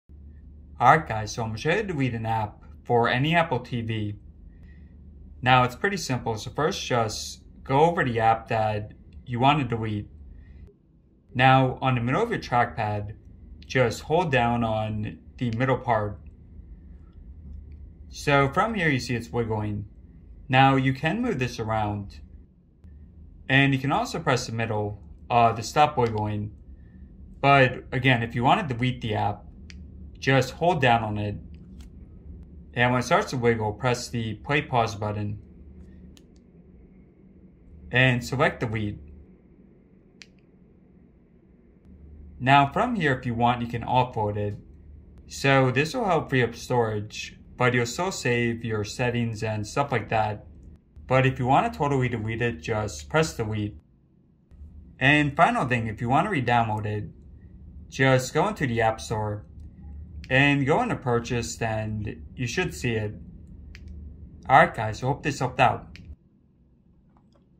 Apple TV 4K How to Delete Apps! (2022 Or ANY Gen)
How to delete apps & uninstall on Apple TV 4K 3rd Gen or any Apple TV..